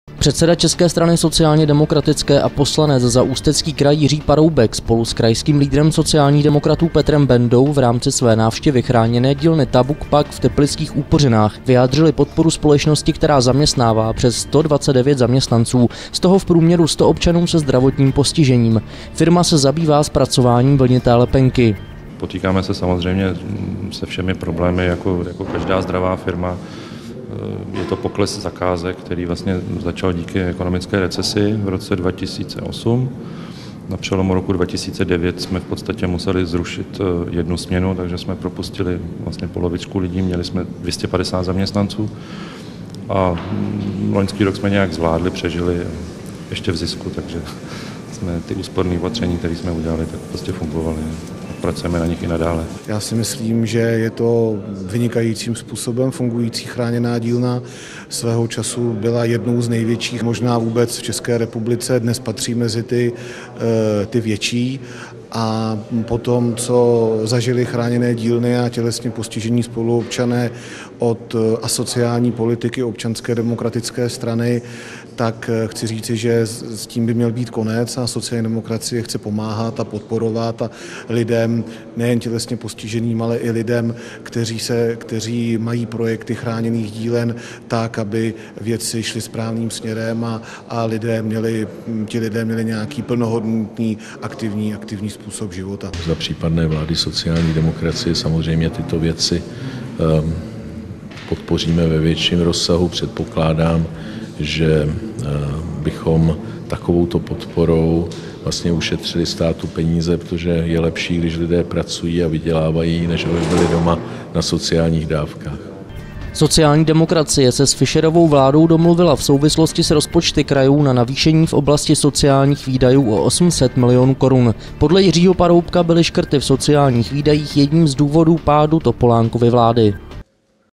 ČSSD: Chráněné dílny potřebují podporu státu i krajů
Předseda ČSSD a poslanec za Ústecký kraj Jiří Paroubek spolu s krajským lídrem sociálních demokratů Petrem Bendou v rámci své návštěvy chráněné dílny TABUC-PACK v teplických Úpořinách vyjádřili podporu společnosti, která zaměstnává přes 129 zaměstnanců, z toho v průměru 100 občanů se zdravotním postižením.